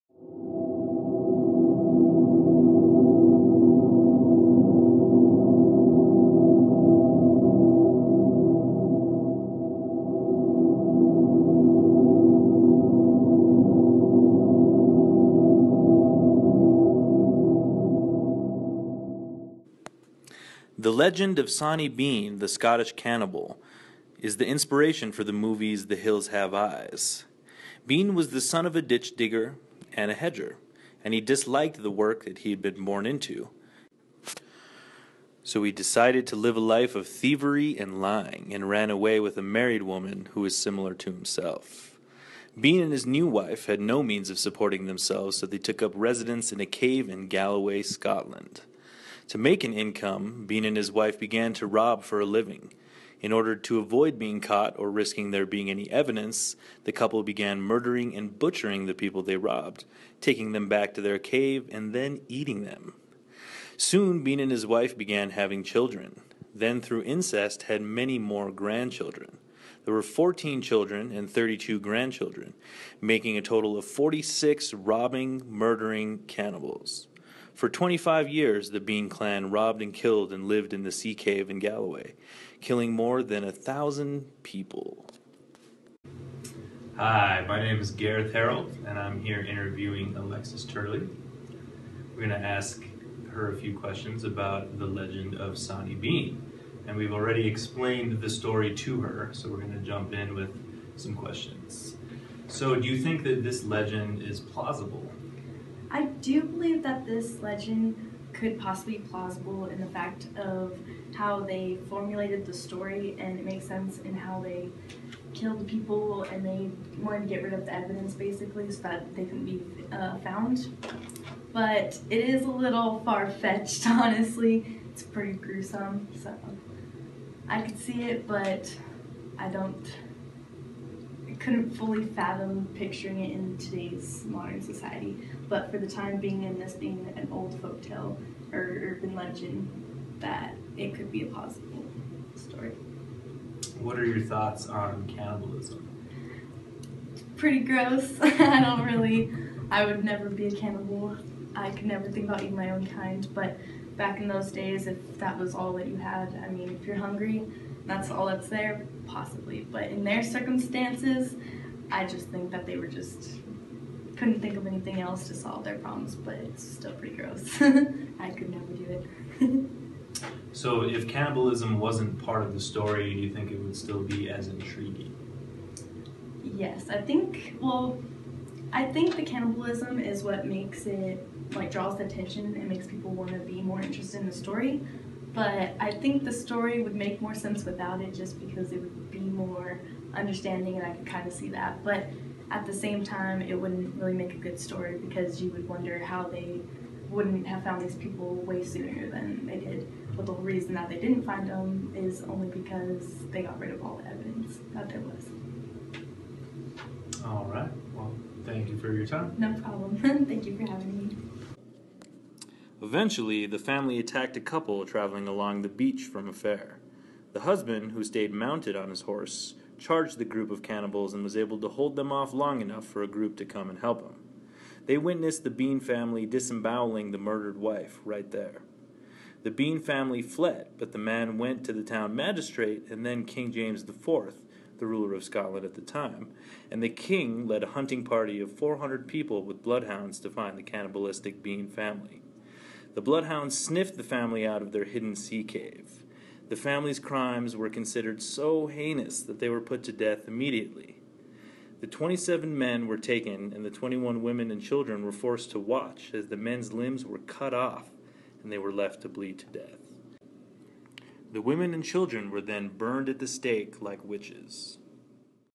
Canabalistic
Documentary rough draft about the legend of Sawney Bean, the Scottish cannibal.